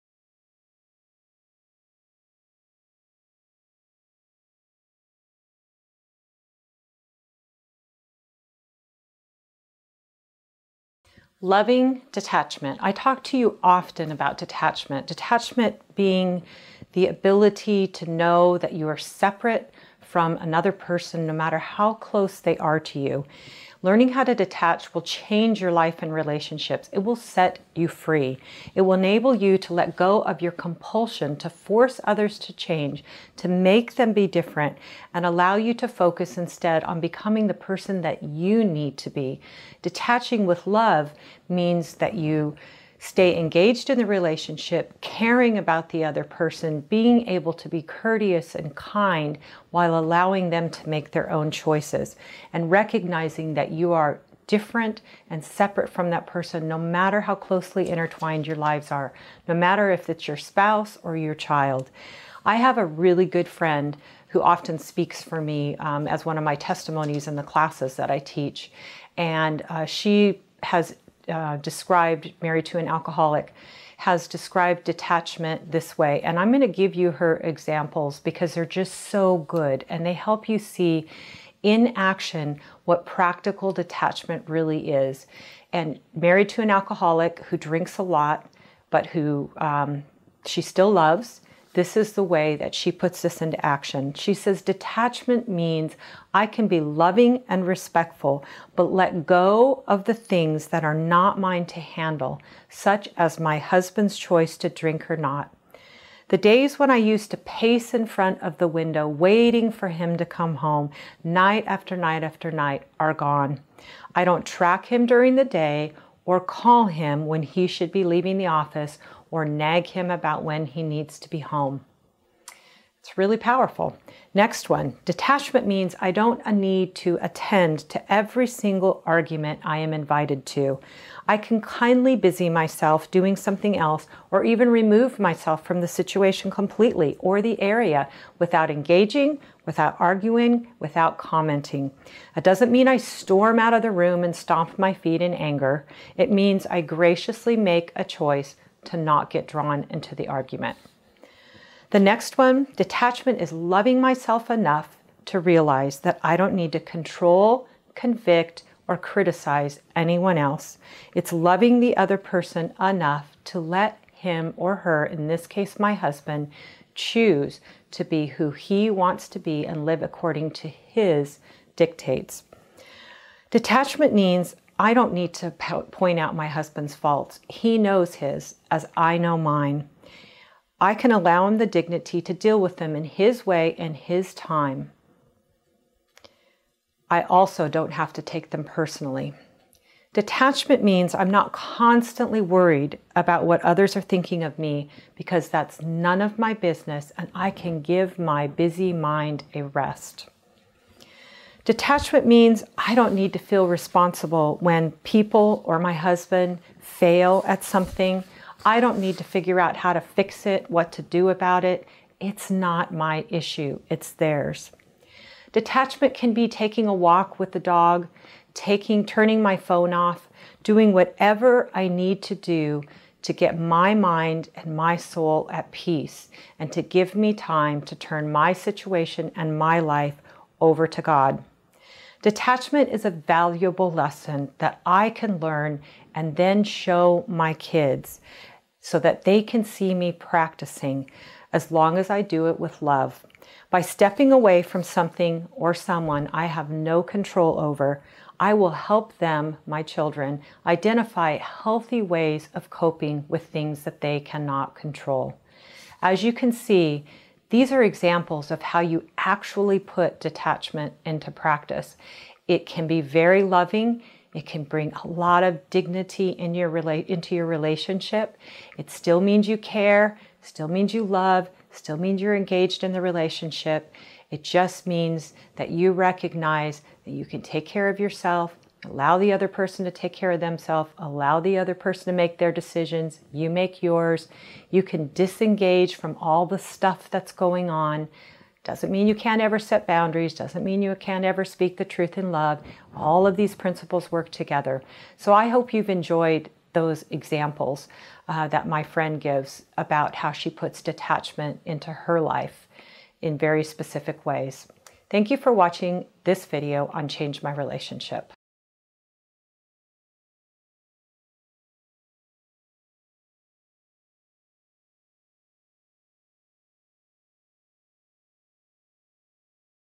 Loving Detachment
It's easy to get entangled in your difficult relationships and lose yourself. Loving detachment allows you to care and let go at the same time.